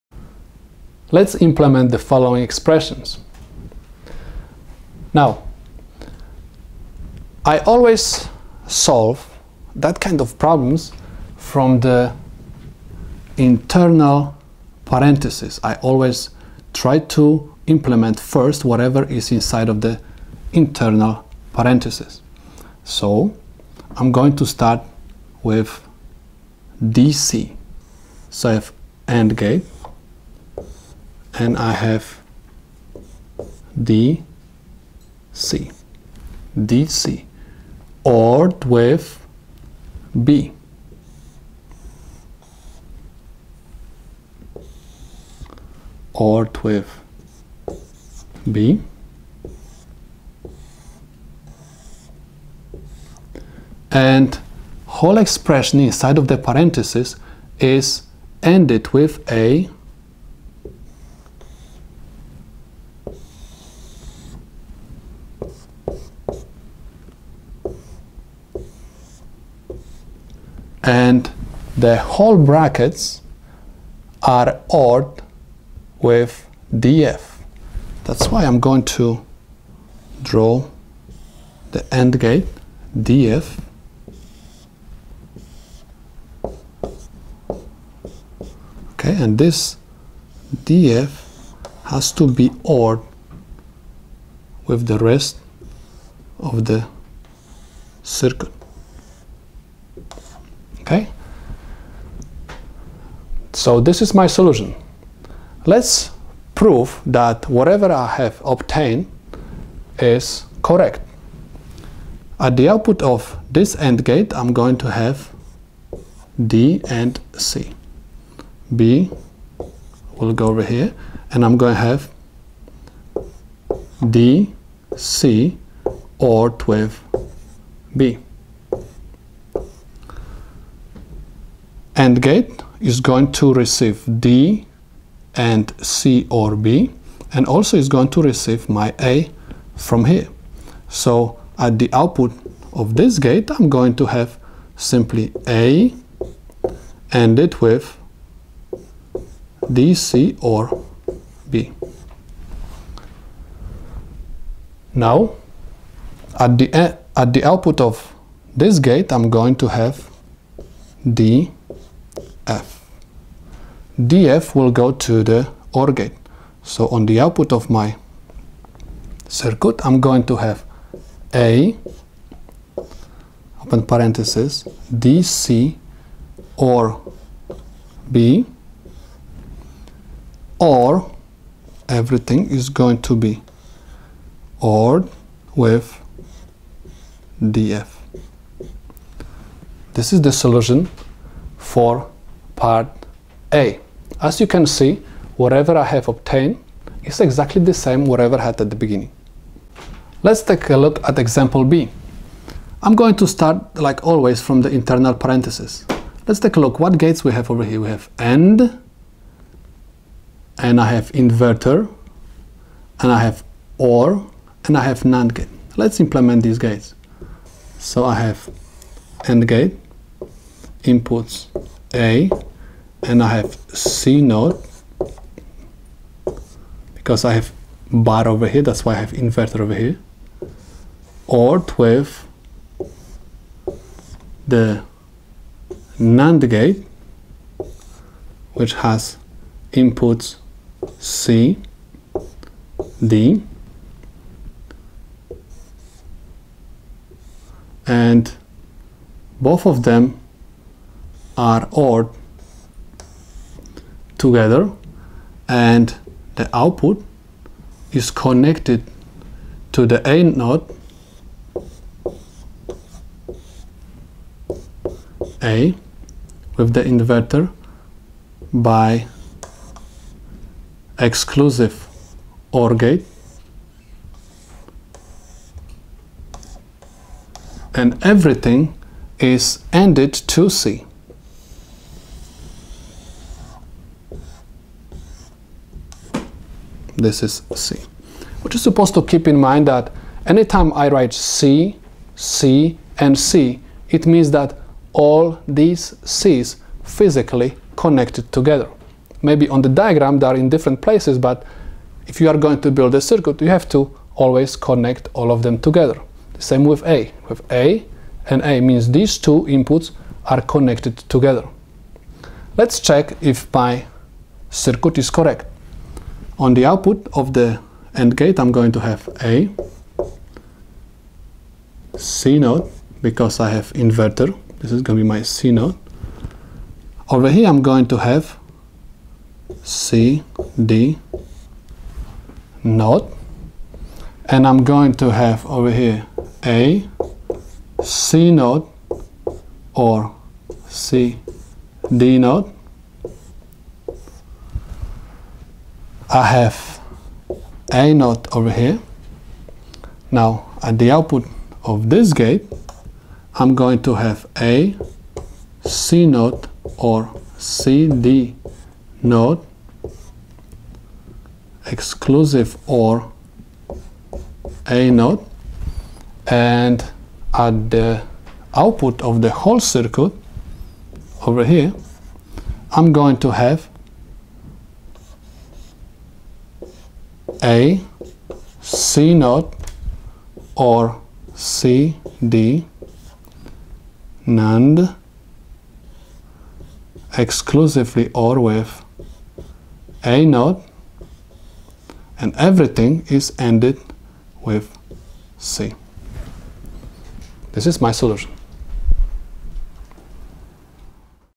Digital Logic - implementing a logic circuit from a Boolean expression. Ex 6
In this video, we explain Combinational Logic and show how to implement given Boolean expressions using logic gates.

This tutorial is perfect for engineering students, ECE, computer engineering, and anyone preparing for GATE or digital logic exams.

📝 Grab a pencil and try the examples with us!